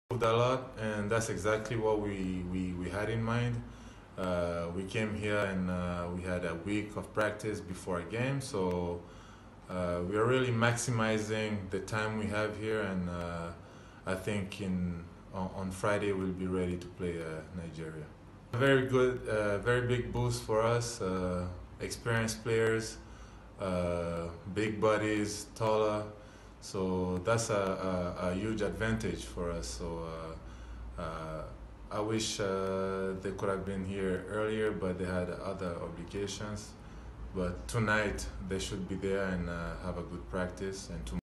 Uganda Basketball team in preparations for FIBA World-cup qualifiers
The men’s senior national team has entered day three of its preparations ahead of the FIBA World Cup qualifiers. The qualifying event tips off on Friday with the Silverbacks taking on Nigeria on Friday in Bamako, Mali. Robinson Opong is confident that Uganda will acquit itself.